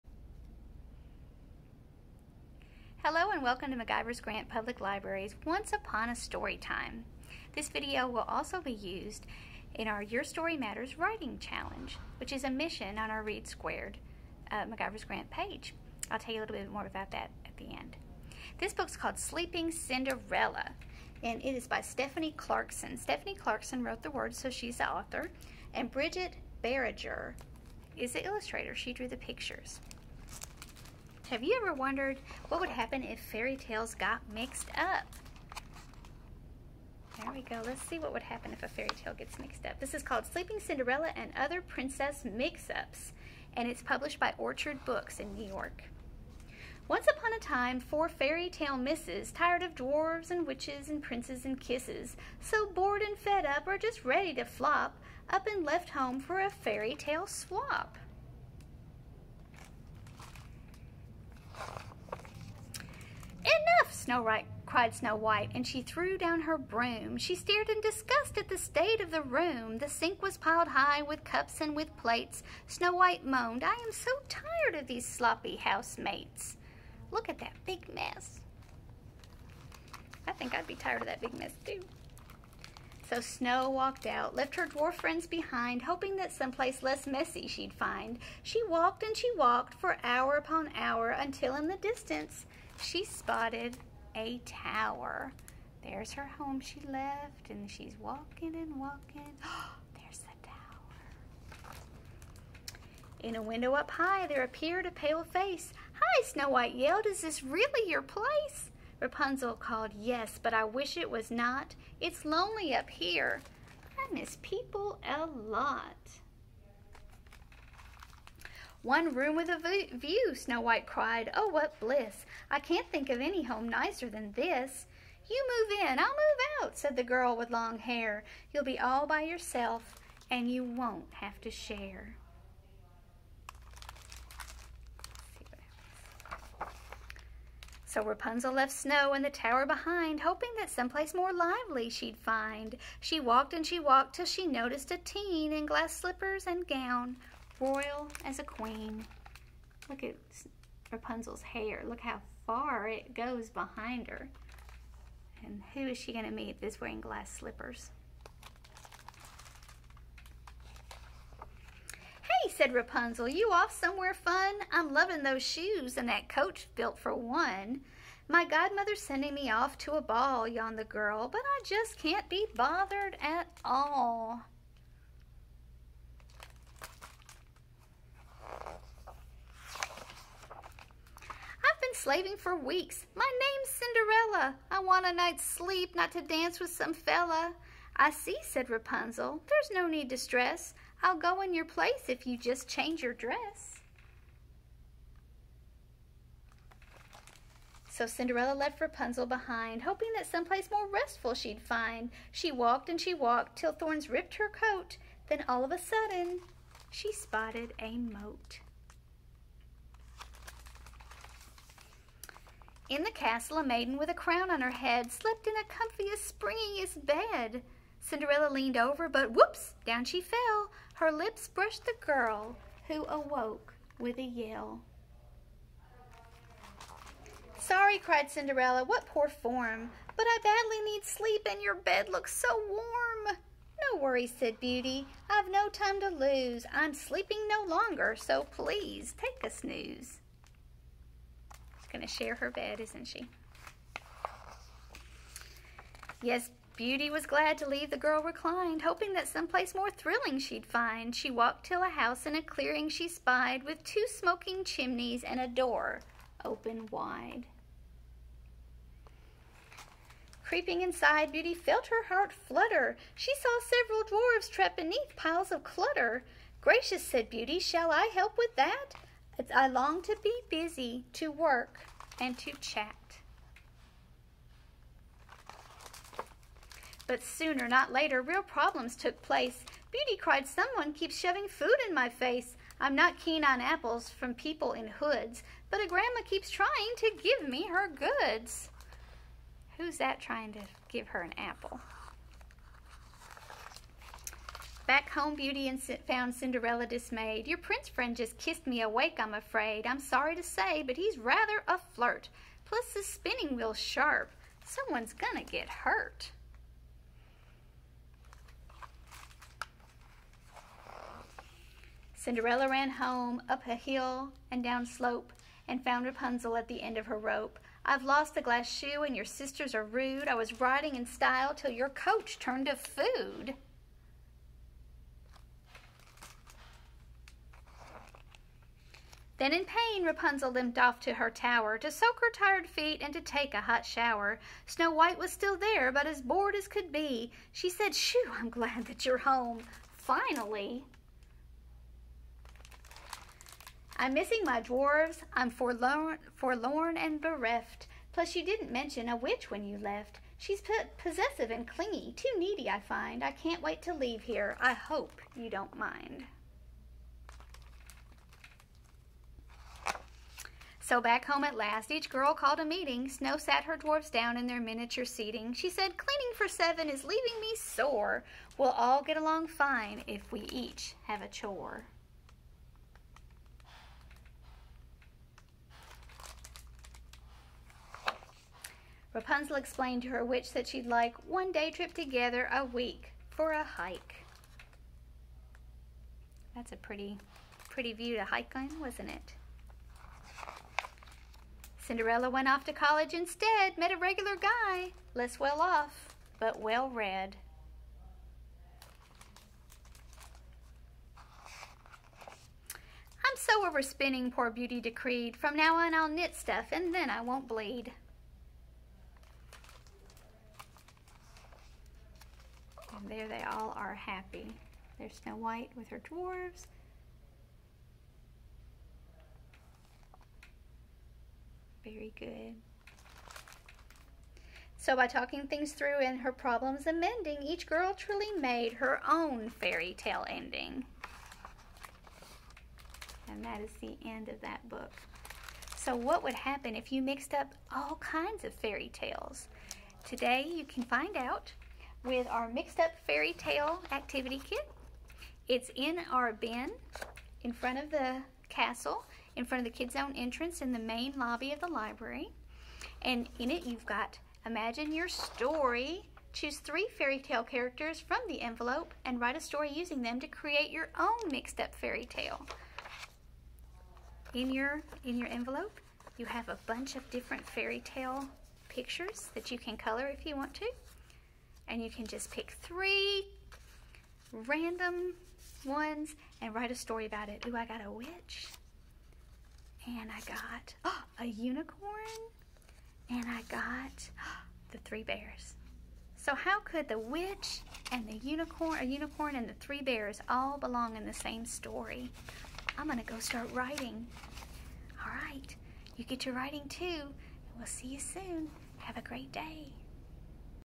Once Upon A Story Time - Sleeping Cinderella
McIver's Grant Public Library's Once Upon a Story Time features Sleeping Cinderella and Other Princess Mix-Ups by Stephanie Clarkson, with illustrations by Brigette Barrager.

This delightful story features a fairy tale mix-up when four fairy tale misses take part in a fairy tale swap. A related activity packet with all the supplies needed to create your own fairy tale mix-up may be picked up from the bin in front of the summer reading display in the main lobby or through the library's curbside service.

Join us today and...
Imagine YOUR Story.